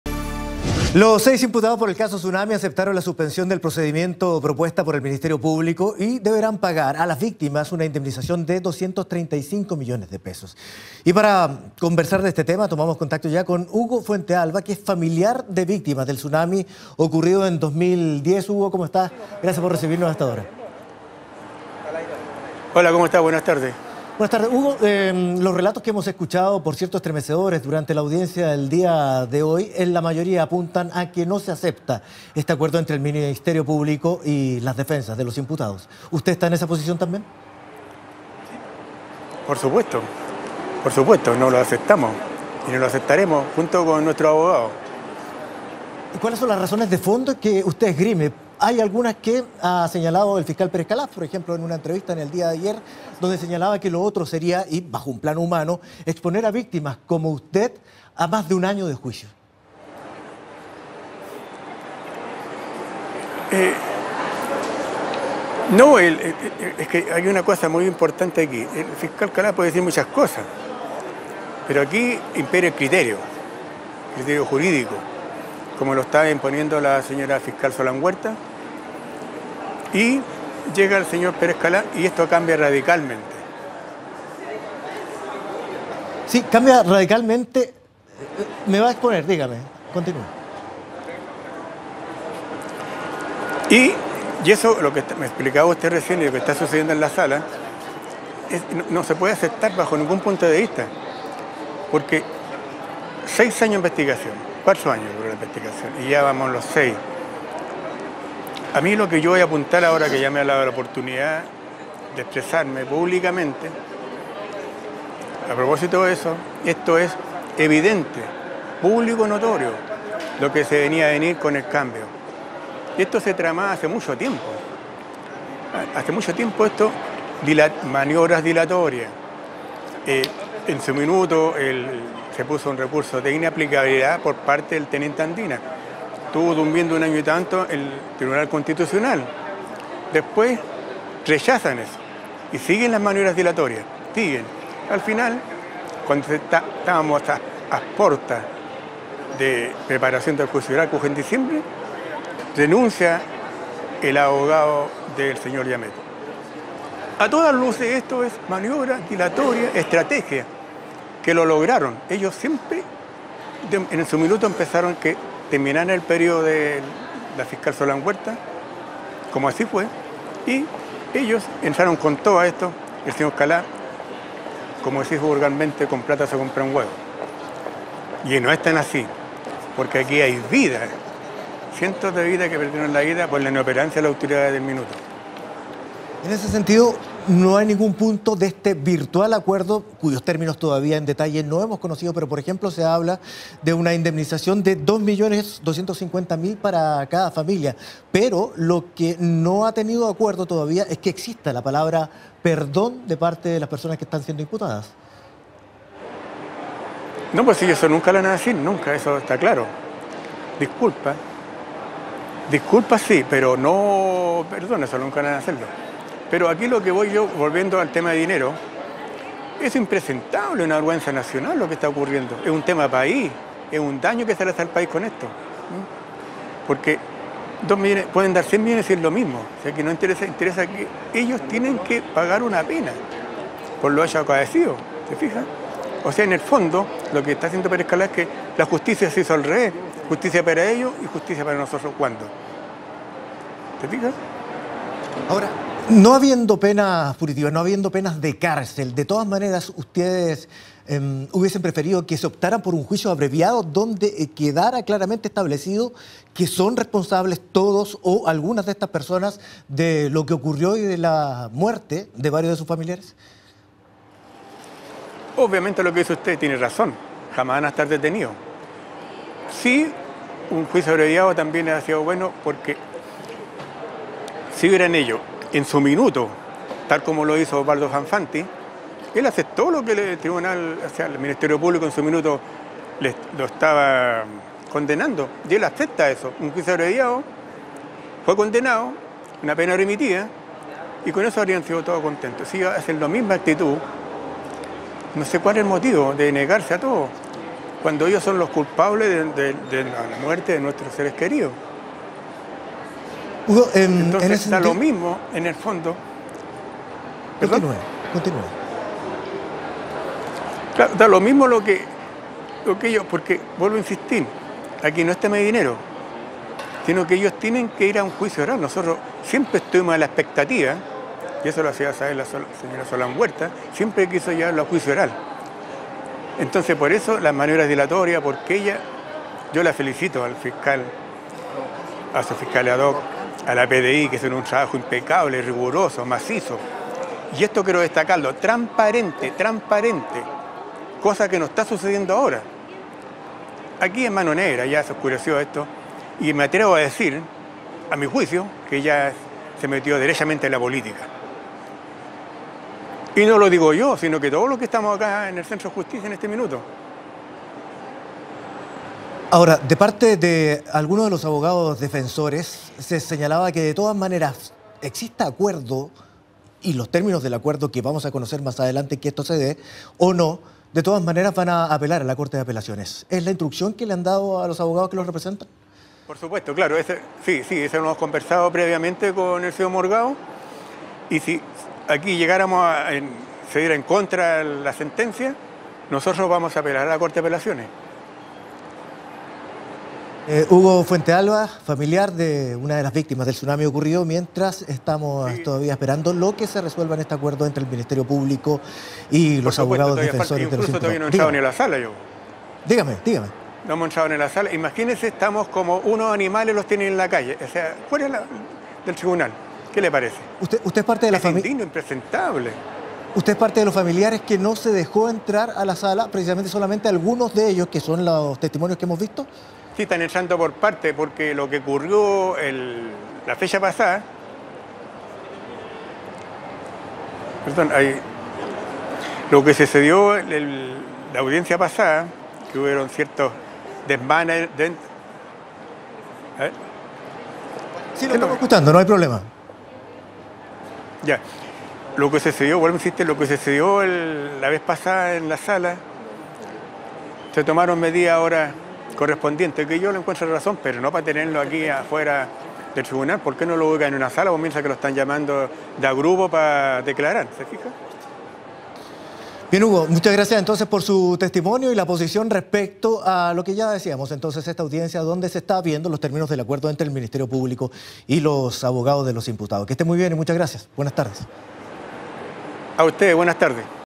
Hugo Fuentealba sobre caso tsunami: "Tienen que pagar una pena"
Los imputados aceptaron el acuerdo para no llegar a juicio oral.